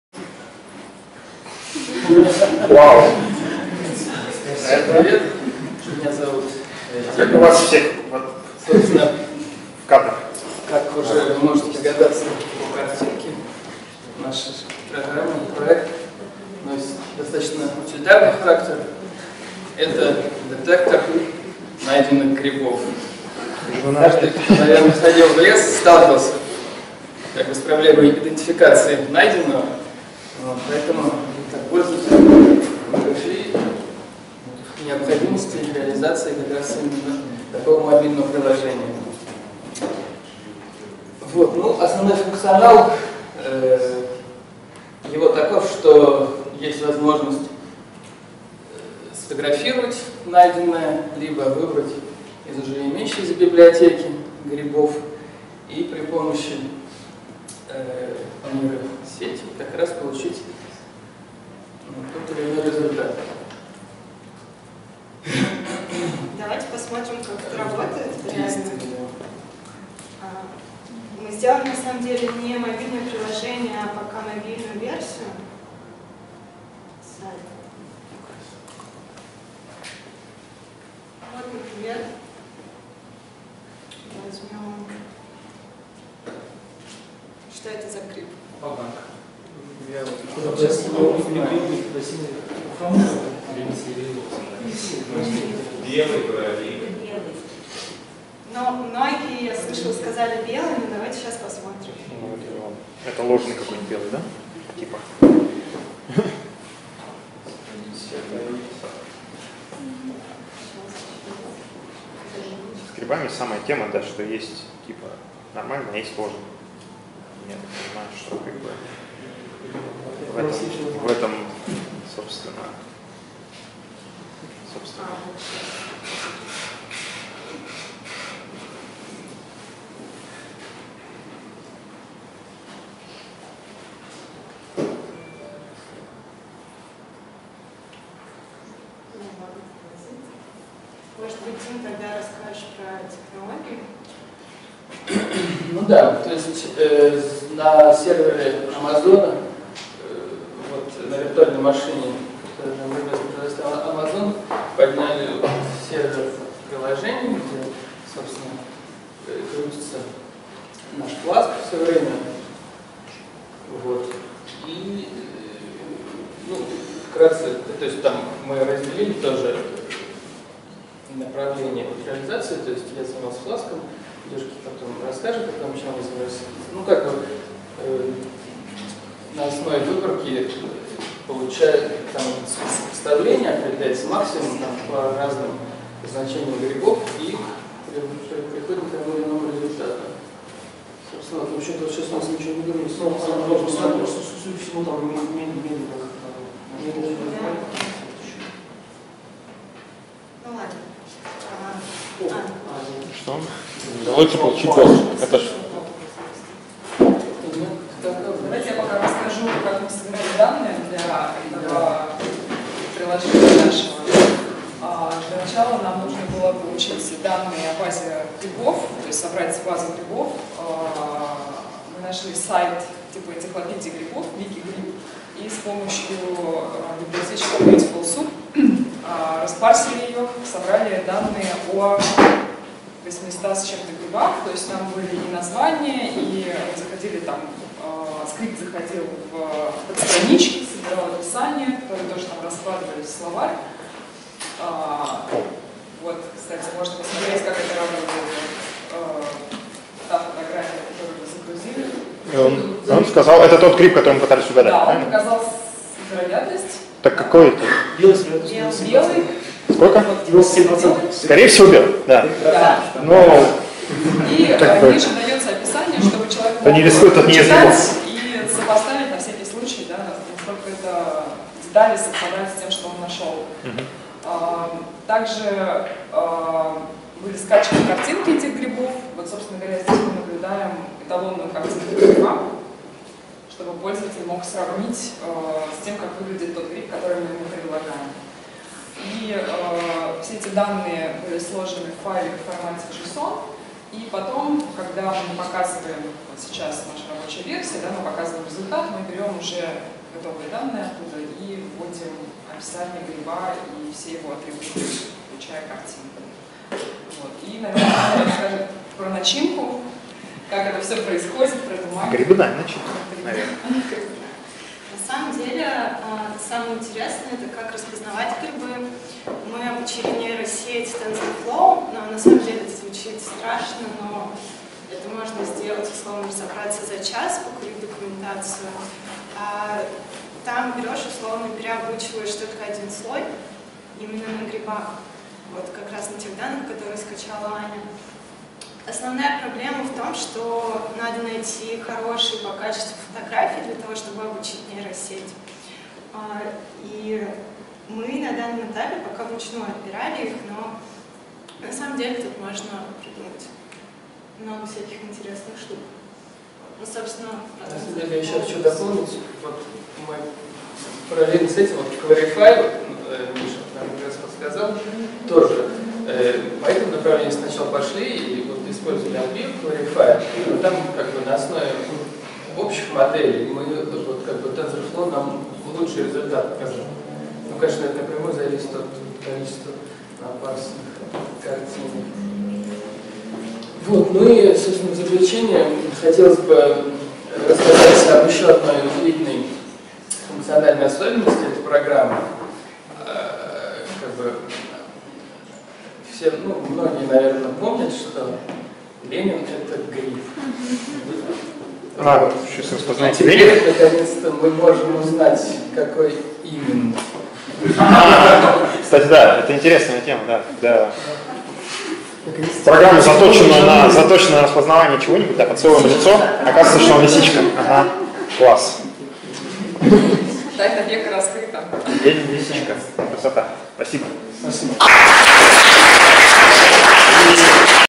Выпуск Learn Python - проект идентификации грибов
Презентация проекта выпускников курса Learn.Python (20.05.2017).